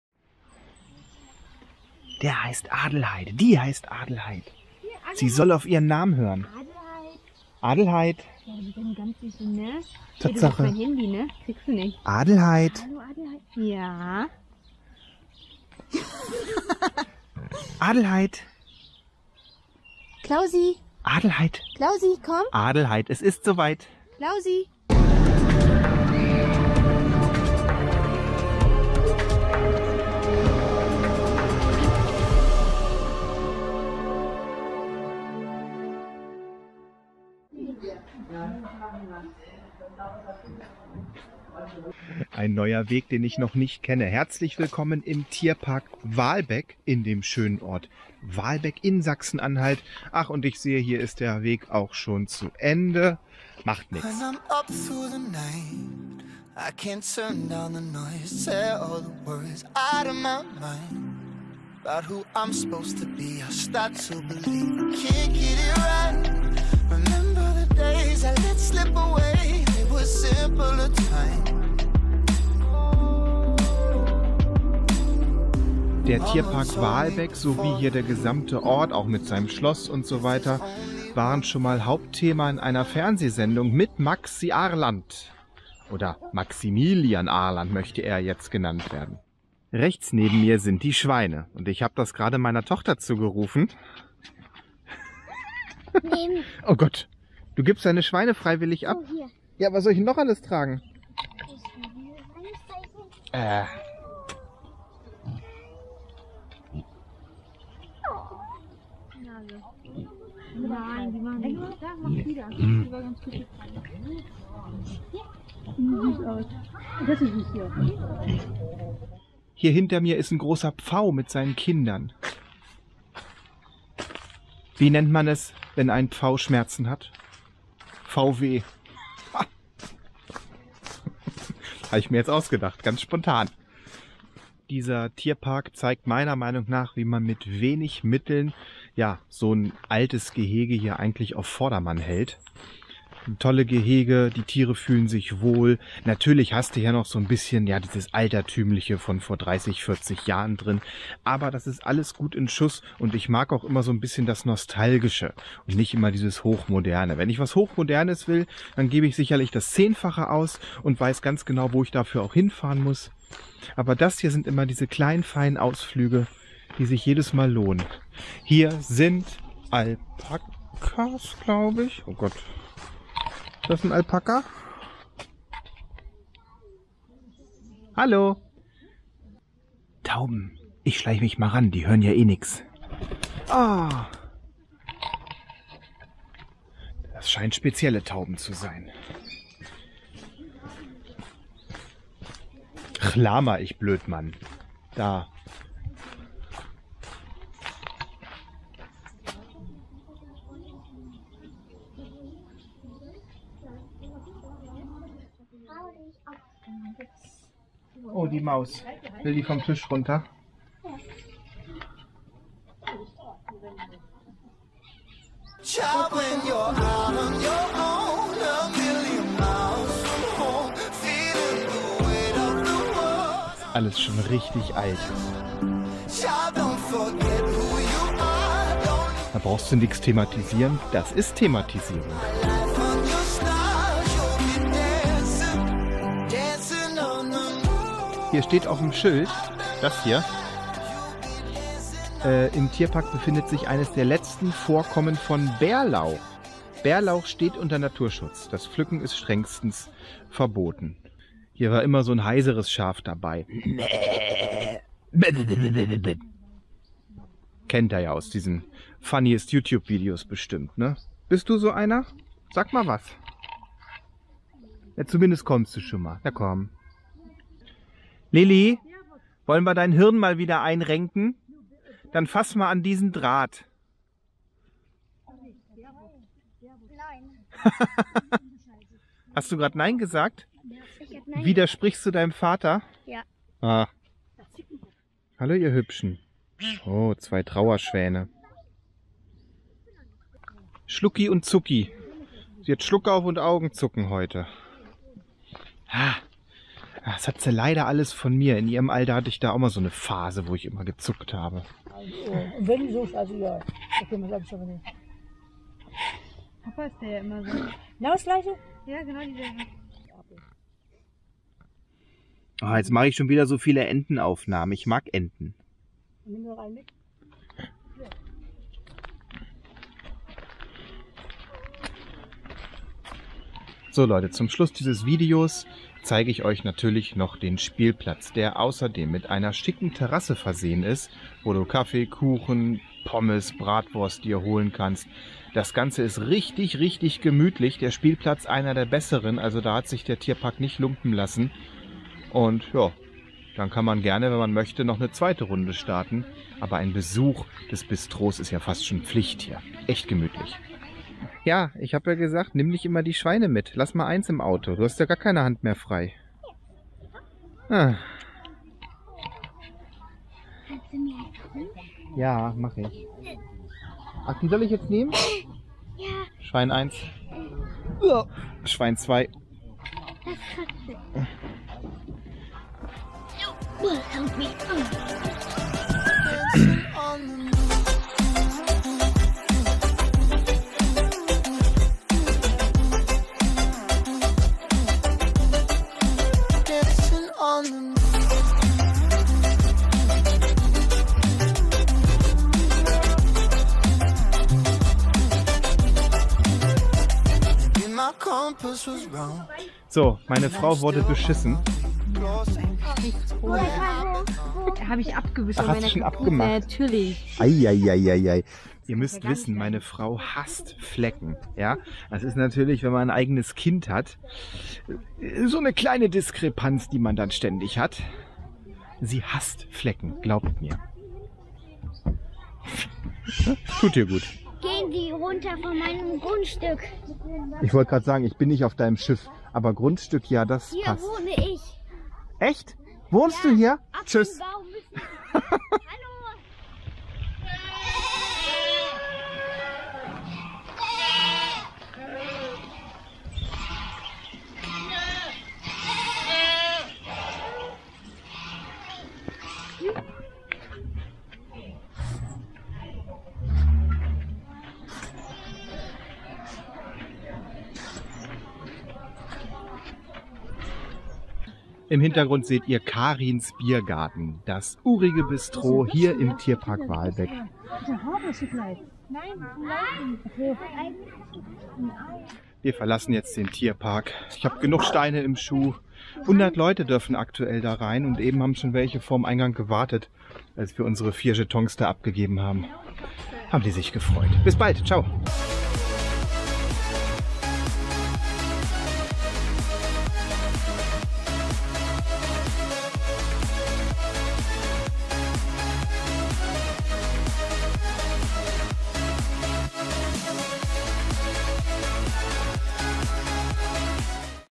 Tierpark Walbeck 2020 - Tolles Ausflugsziel im Ostharz
Ich war bereits zum 5. Mal in diesem kleinen, feinen und äußerst sympathischen Tierpark - und ganz sicher nicht zum letzten Mal! Im Tal des Ortes Walbeck gelegen überzeugt diese Anlage durch Naturbelassenheit, Idylle, Gemütlichkeit, Freundlichkeit, Weitläufigkeit, gepflegte Tiere sowie den schönen Spielplatz und den urigen Biergarten. Ein tolles Ausflugsziel in der Nähe von Hettstedt und Aschersleben. Viel Spaß beim Zuschauen!